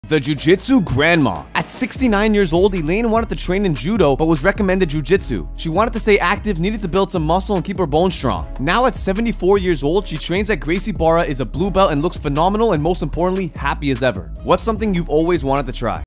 JIU JITSU GRANDMA!
This is the Jiu Jitsu grandma! She started learning at 69 and now at 74 she's a blue belt and happy as ever!